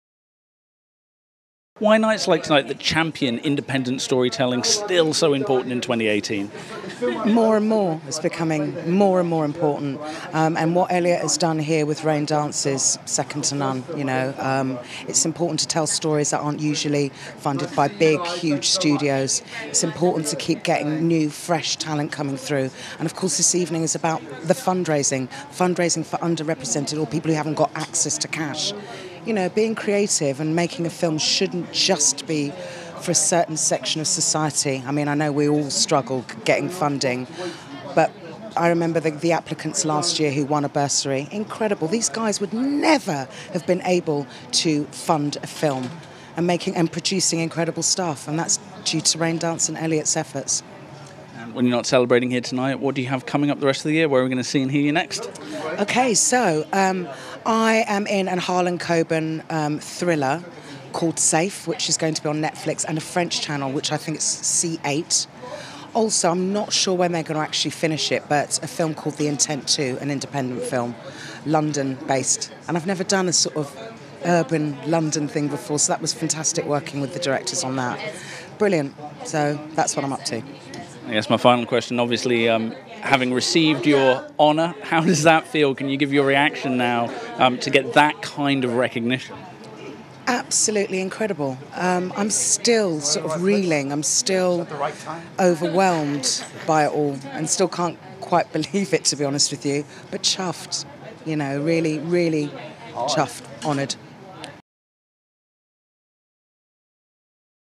Karen Bryson Interview Raindance Independent Filmmaker's Ball 2018
Red Carpet News TV talks to VIP guests at the Raindance Independent Filmmaker's Ball 2018 in London. VIP interviews include Jeany Spark, Jimmy Akingbola, Karen Bryson, Christian Brassington, Kellie Shirley, Christian Vit, Pasha Bocarie, Kiri Bloore, Amar Adatia, Ronan Summers, Sean Cronin Rosie Fellner, Simona Roman, Mark Stothert and Jess Impiazzi.